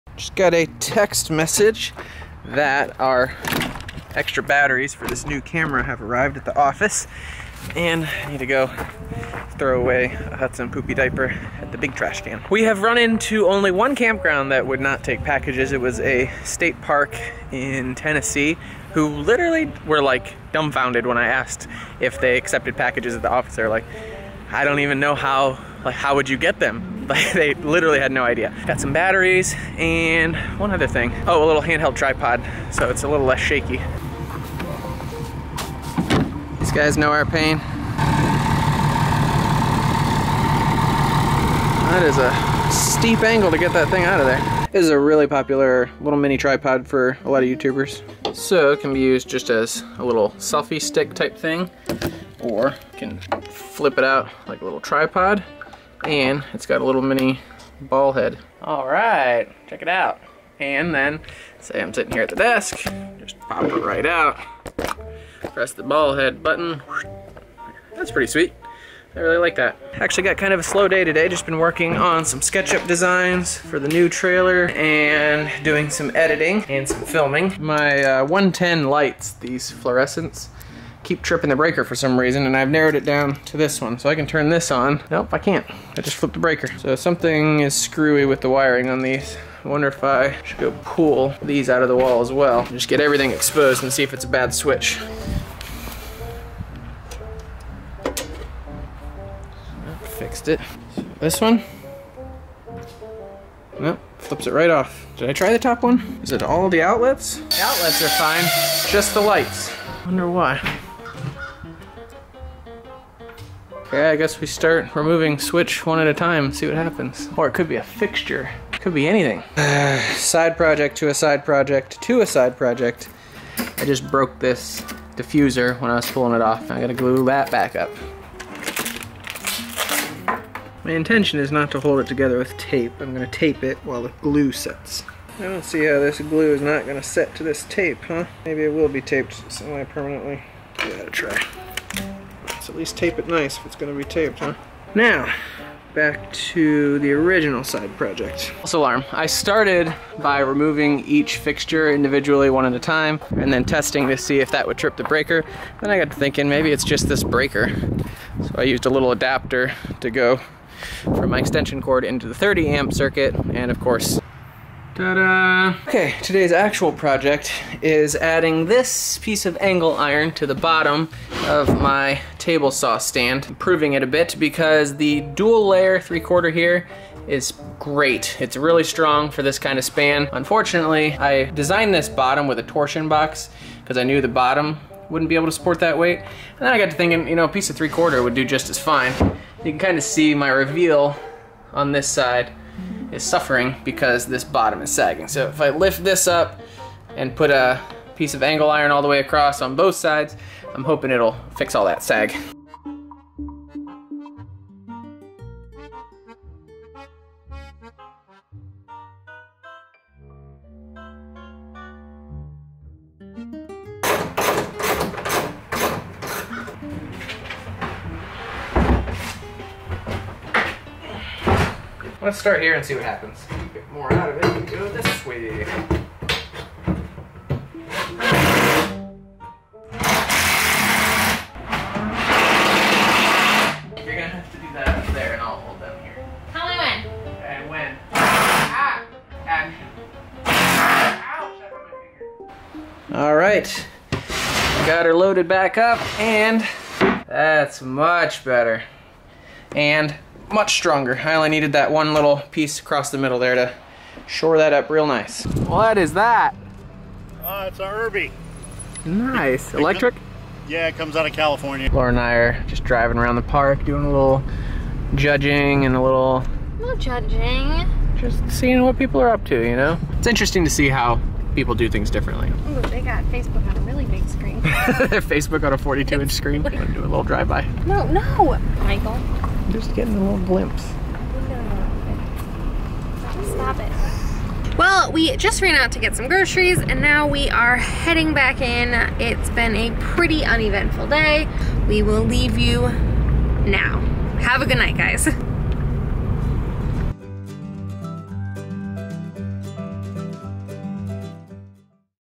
VLOG | Improving the Table Saw Cabinet (Day 21 of 24)
We've decided we're going to vlog our transition into full time RV living for the next 30 days.  In this episode we troubleshoot the lighting in the mobile workshop and improve the table saw cabinet design.

Follow along as we drive our 1999 Monaco Dynasty towing a 24 foot enclosed trailer (Ultimate Mobile Workshop) around the country helping viewers with their DIY projects!